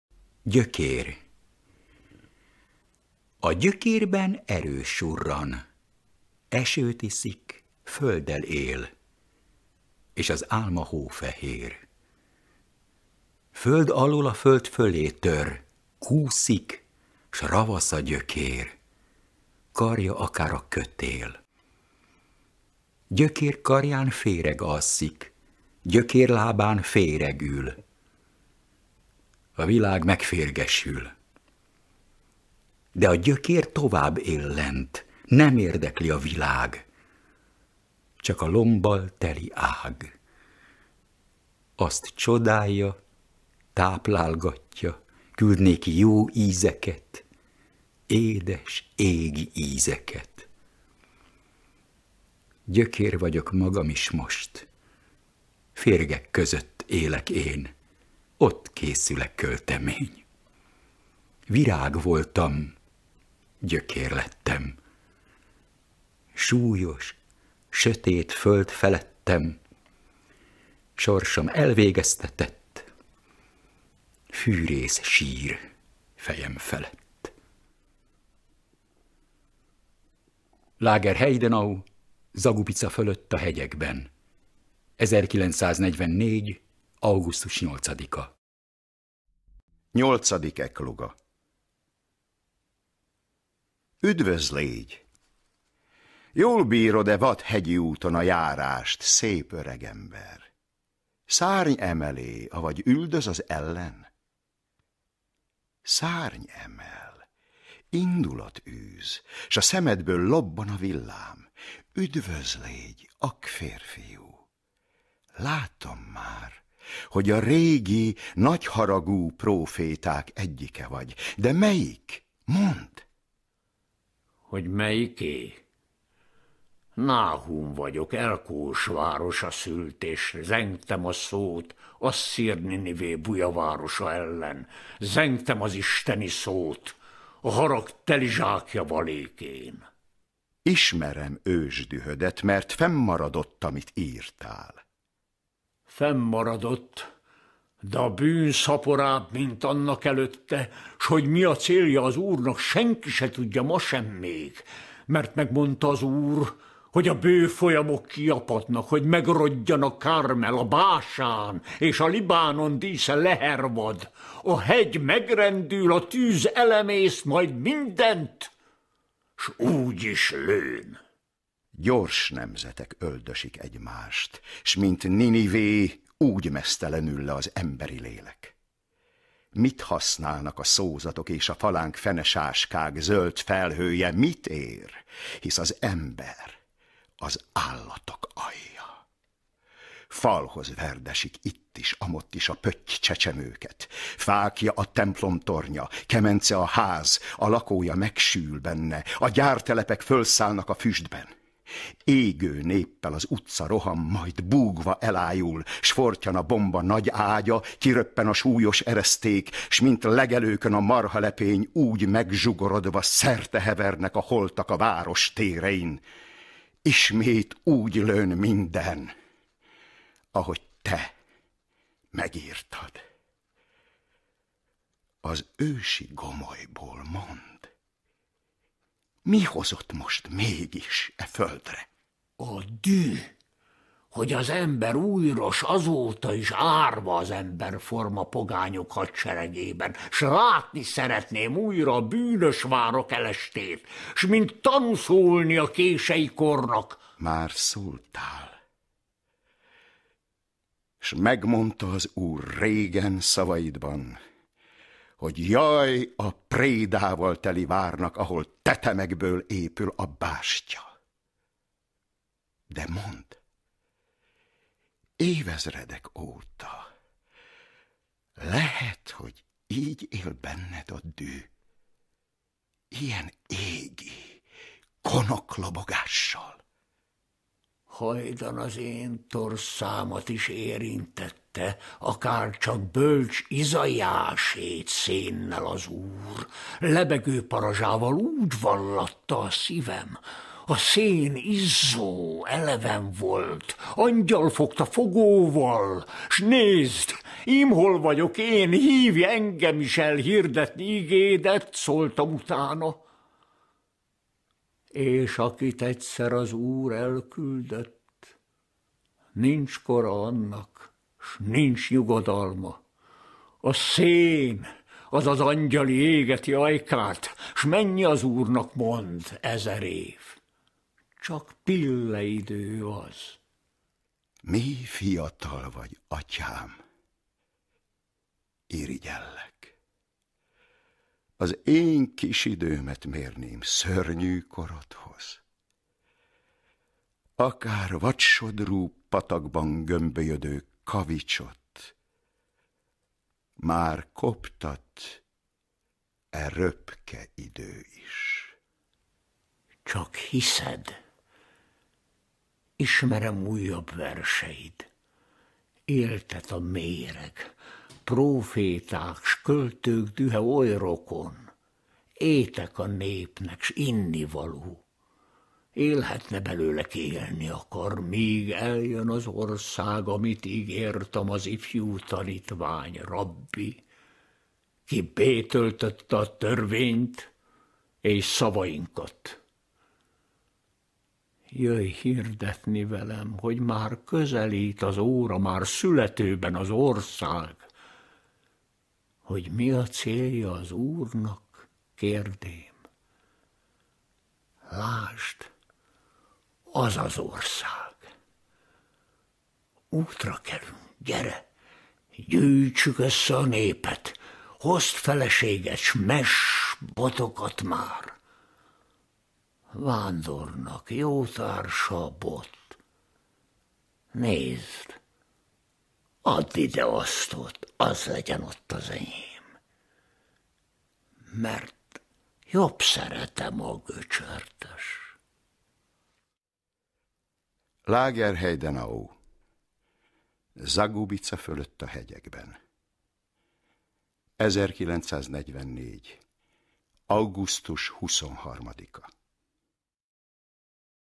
75 ÉVE EZEN A NAPON GYILKOLTÁK MEG RADNÓTI MIKLÓST
75 ÉVE EZEN A NAPON GYILKOLTÁK MEG RADNÓTI MIKLÓST
(Abda, 1944. november 9)

Két megrázó versével emlékezzünk rá:
Radnóti Miklós: Gyökér – Garai Róbert
Radnóti Miklós: Nyolcadik ecloga – Hegedűs D. Géza és Kézdy György

Radnóti Miklós születésének 100. évfordulója alkalmából készült irodalmi CD két verse hallható.